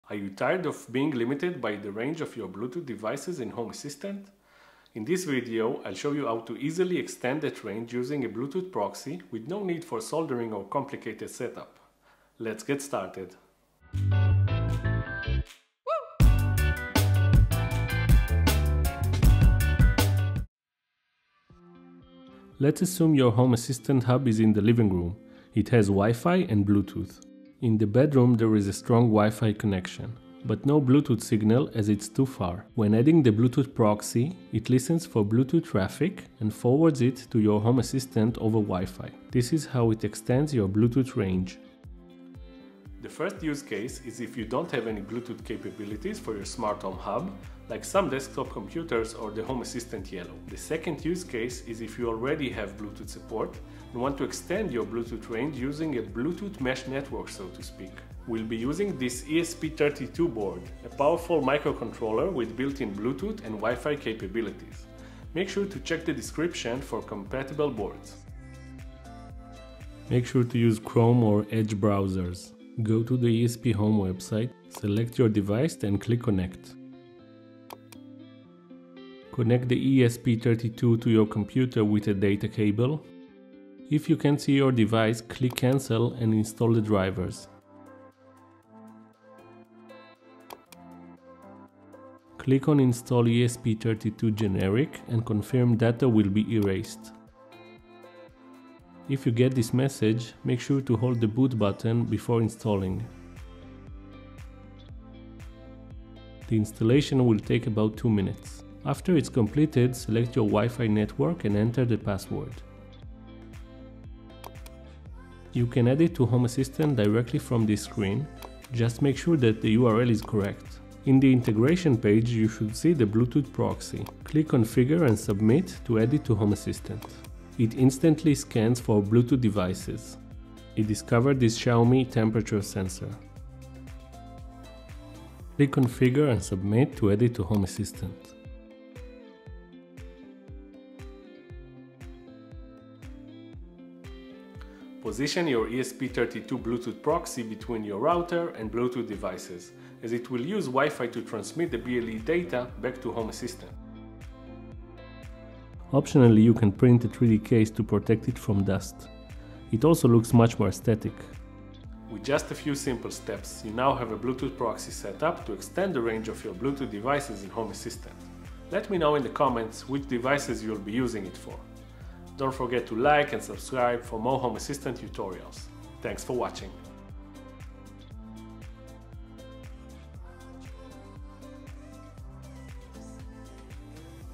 EASY Bluetooth Proxy In Home Assistant ESPHome- HOW TO | ESP32
In this video we will set up a Bluetooth Proxy in Home Assistant using an ESP32 development board

ESP32 Board:
Amazon US
Amazon Germany
Amazon UK

M5Stack Atom Lite:

00:00 - Intro
00:21 - What is a Bluetooth Proxy?
00:42 - Use Cases For a Bluetooth Proxy
00:58 - Required Hardware
01:08 - Installing ESPHome Bluetooth Proxy
01:39 - Installation Failed?
01:55 - Adding Bluetooth Proxy To Home Assistant
02:09 - Adding a Bluetooth Device
02:24 - Where To Place The ESPHome Bluetooth Proxy?
02:34 - 3D Printed Case For The ESP32